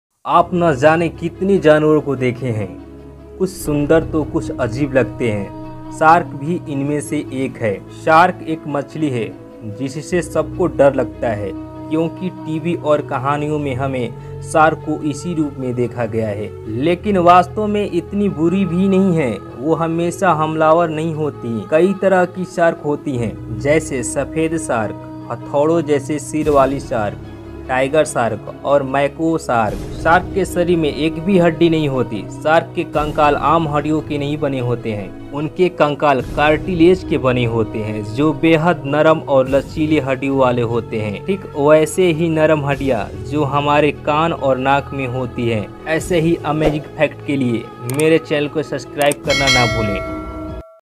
इतना बड़ा जानवर और हड्डियाँ केवल नाम मात्र जानो दिलचस्प Fact🔥 //
इस वीडियो में सबसे कम हड्डियों वाला जानवर का नाम बताया गया है ।।
इसमे शर्क के बारे में बात किया गया है कि शार्क कितने प्रकार के होते है ।।और कौन जानलेवा हो सकते है ।।

1).सफेद शार्क

2).हथौड़ों जैसे सिर वाली शार्क

3).टाइगर शार्क

4).मैको शार्क